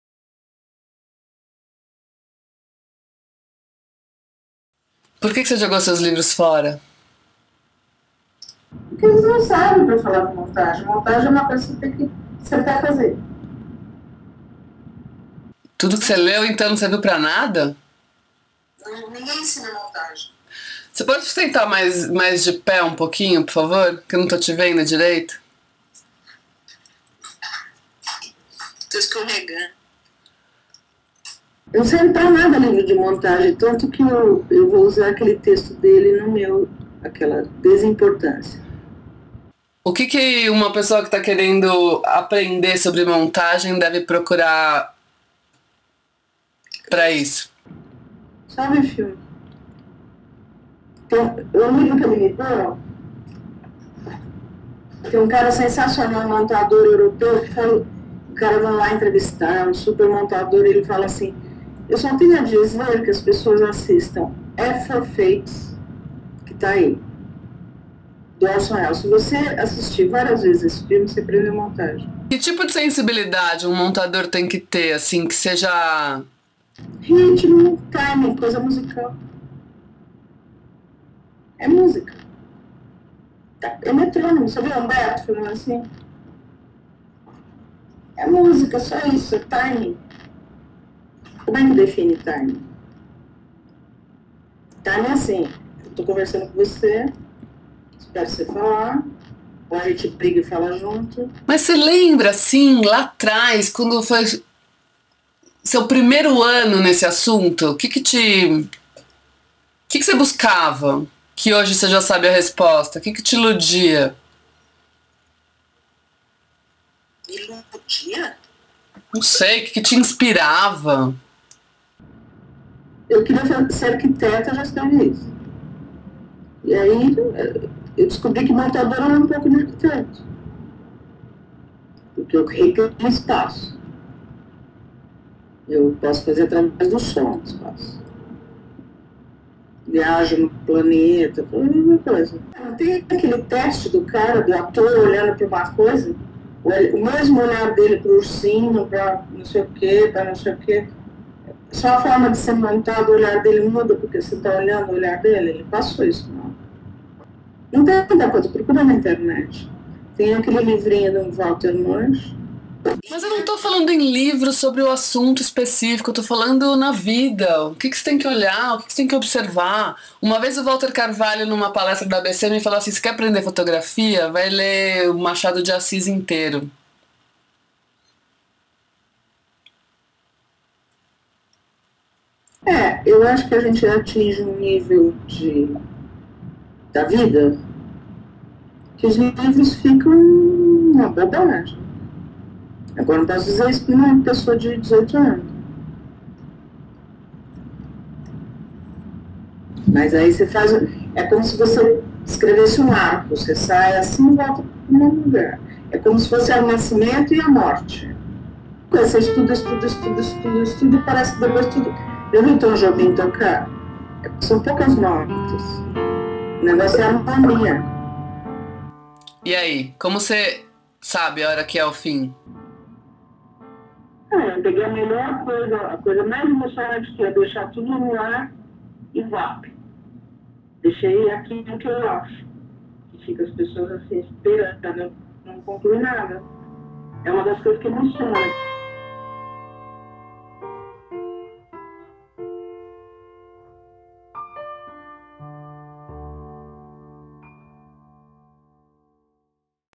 MONTAGEM PAPO NO SKYPE (Skype Me)
Papo no Skype. Pega de surpresa, Camila conseguiu tirar algumas idéias. Depois de um tempo, resolvi editar. Vamos falar de montagem e edição de cinema.